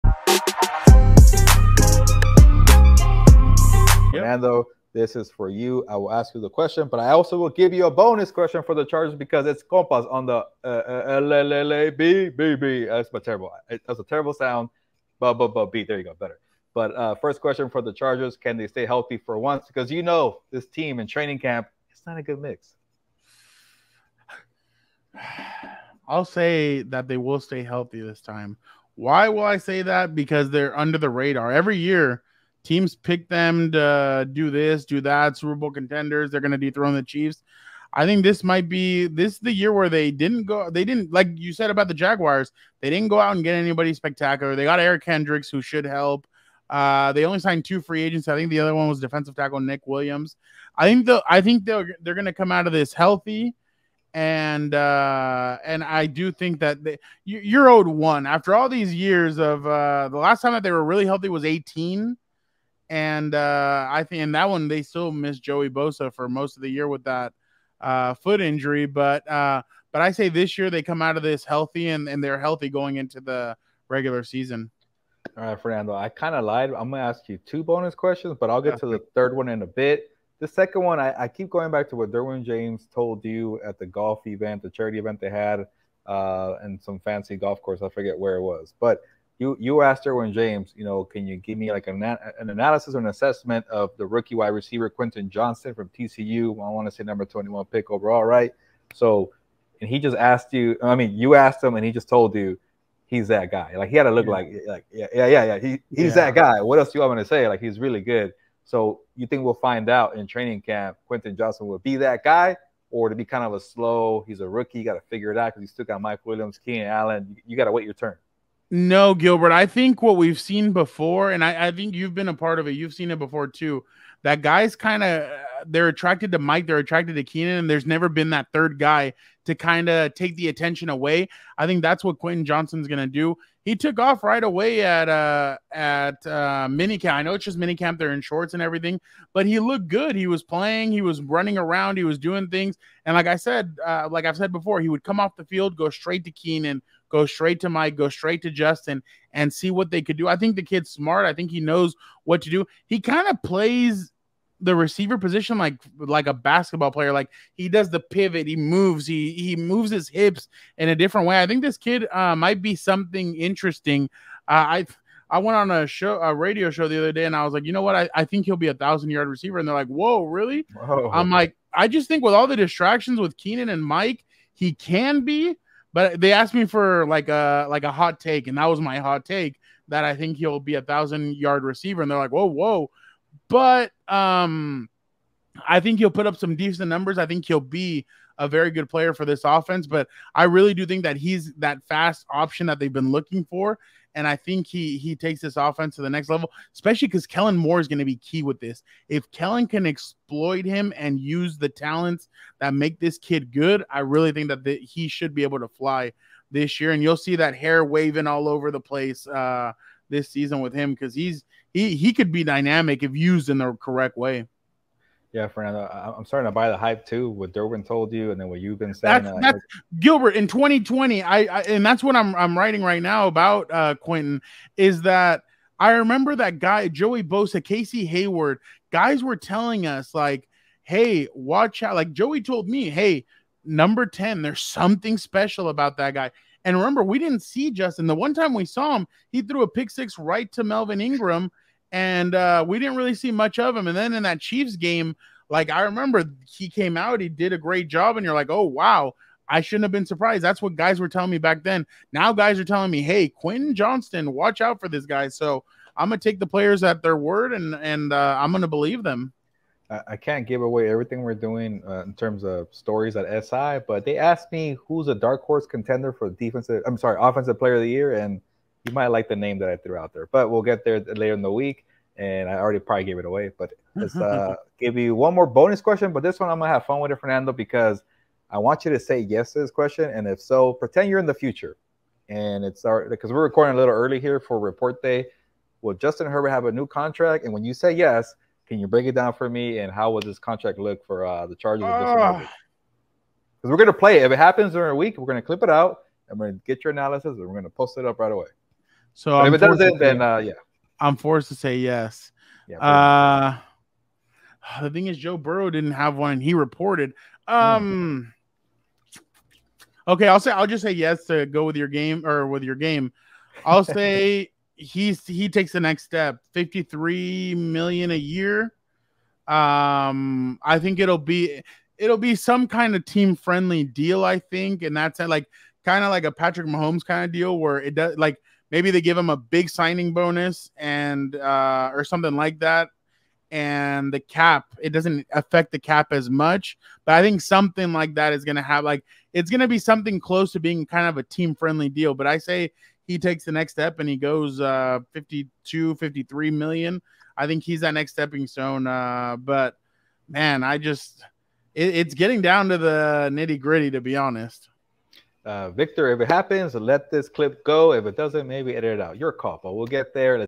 Chargers training camp preview: Justin Herbert’s new deal & Quentin Johnston’s fast start
Gilberto asks Fernando three important questions pertaining to the Los Angeles Chargers heading into training camp. Including what will a Justin Herbert contract extension look like? Fernando has the answers while Gilberto judges his responses.

ALL COMPAS CONTENT IN ONE PLACE

FOLLOW THE COMPAS ON SOCIAL